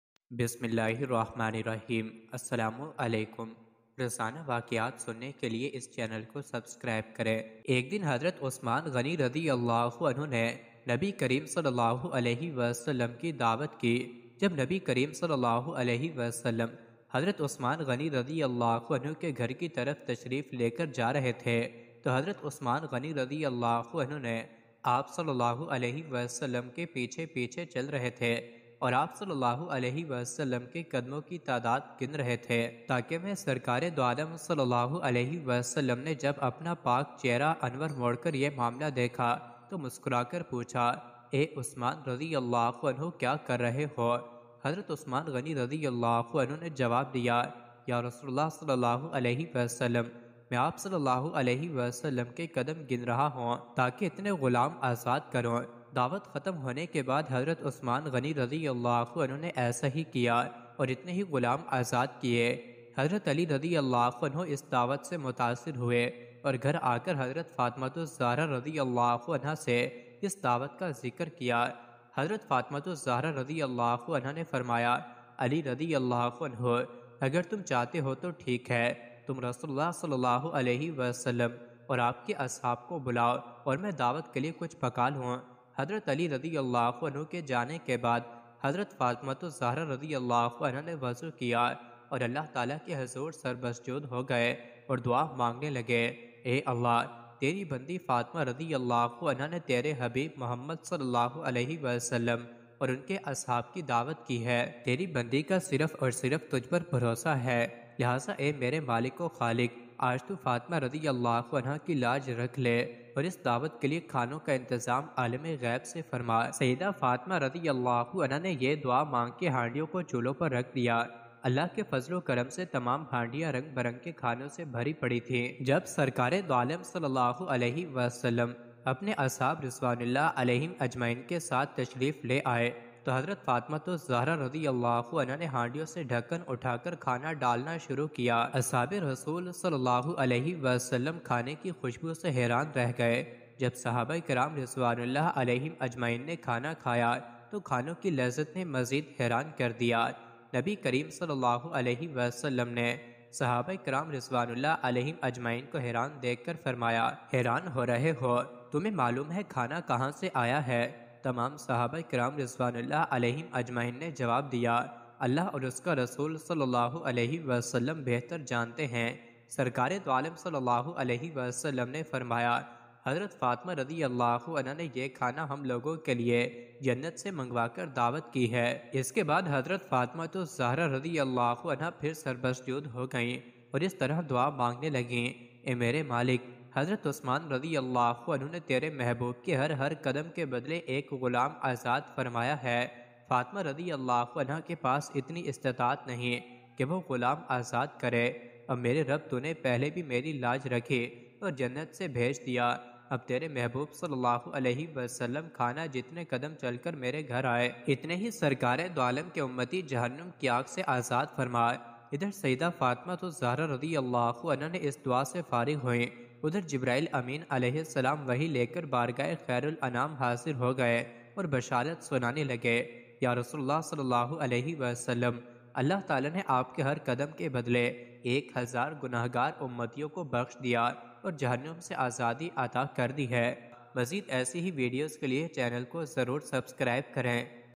Story Of Hazrat Usman (R.A) And Hazrat Ali (R.A) | Hazrat Ali (R.A) Ka Dawat Deny Ka Waqia
One day, Hazrat Uthman invited the Prophet (peace be upon him) by which Hazrat Ali was very impressed.There was nothing, suddenly it happened that everyone was surprised. ?